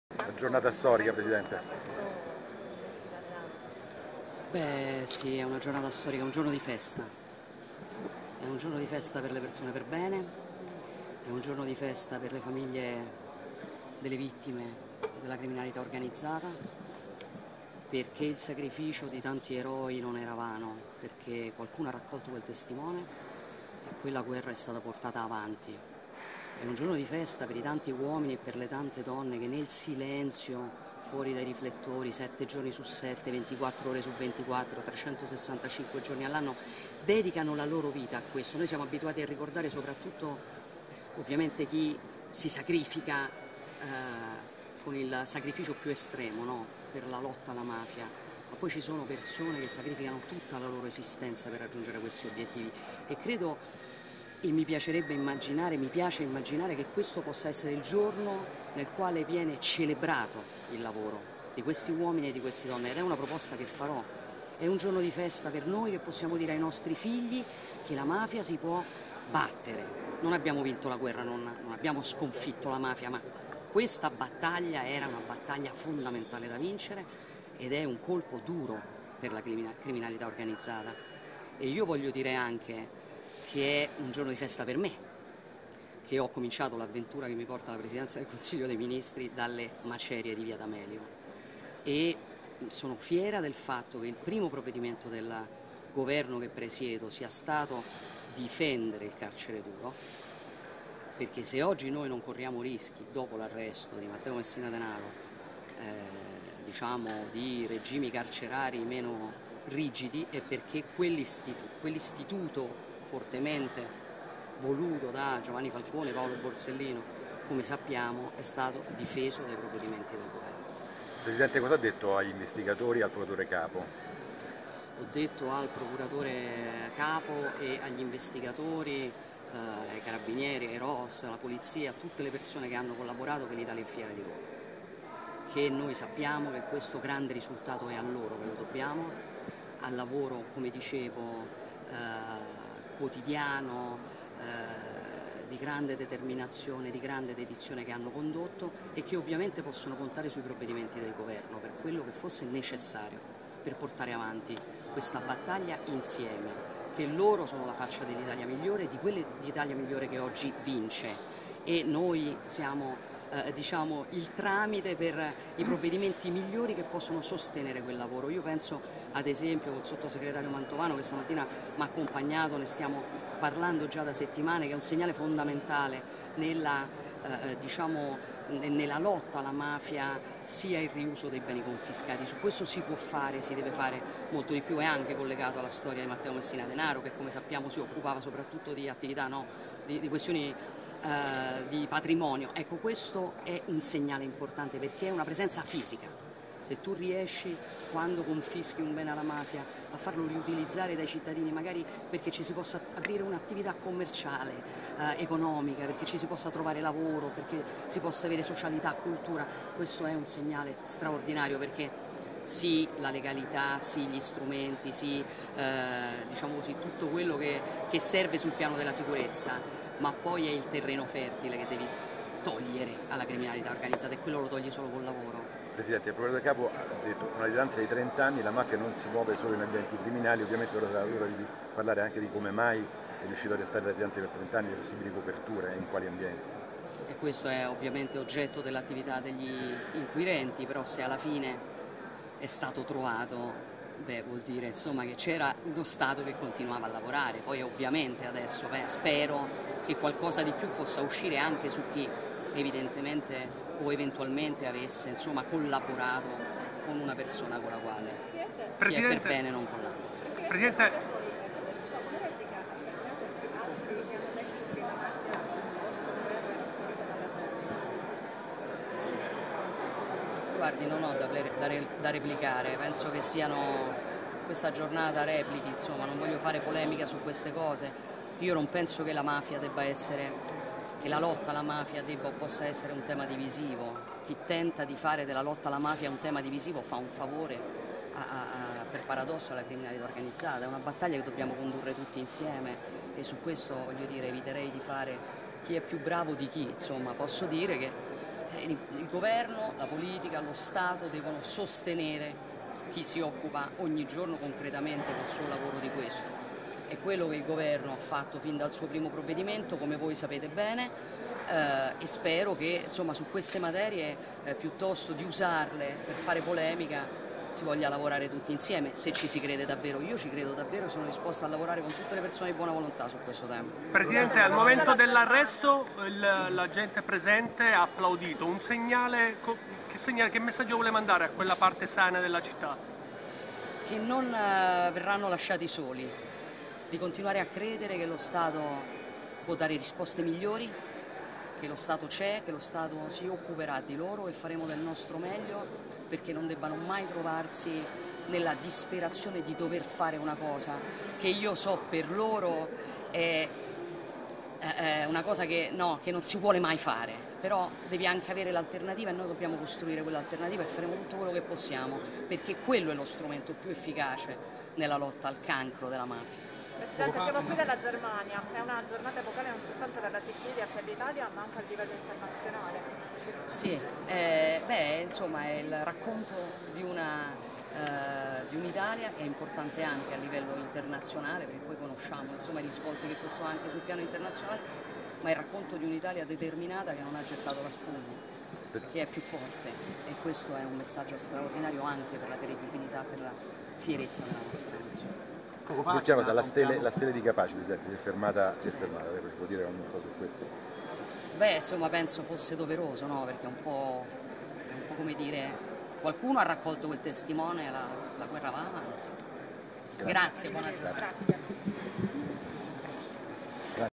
Arresto di Matteo Messina Denaro, punto stampa del Presidente Meloni a Palermo
Palermo, 16/01/2023 - Punto stampa del Presidente del Consiglio Giorgia Meloni, a Palermo per incontrare il procuratore distrettuale di Palermo, Maurizio de Lucia, i magistrati che hanno coordinato le indagini e i carabinieri del Ros che hanno eseguito l'arresto del boss super latitante Matteo Messina Denaro.